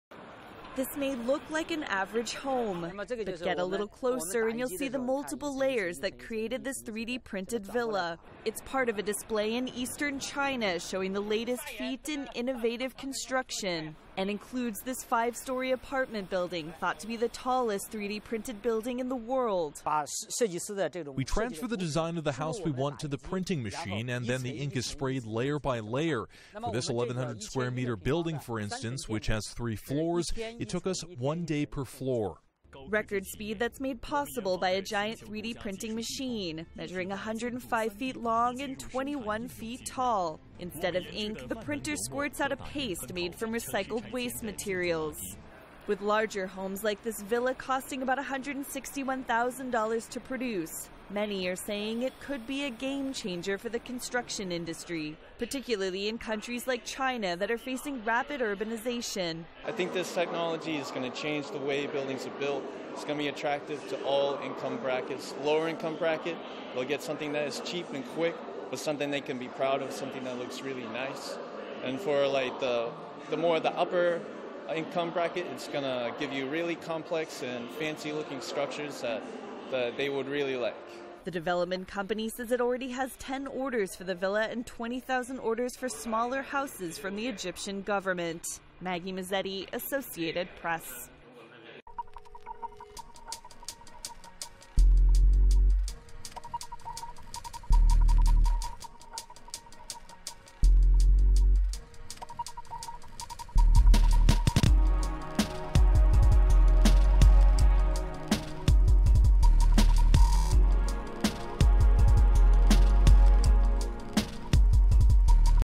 Maisons Imprimées : La Chine Révolutionne le Monde Du Bâtiment!
Après la Grande Muraille, les grandes maisons imprimées! Découvrez dans ce reportage de l'Associated Press comment la Chine compte bouleverser le monde de la construction! Plus d'infos dans notre article

Retrouvez nos produits pour la recharge jet d'encre et toner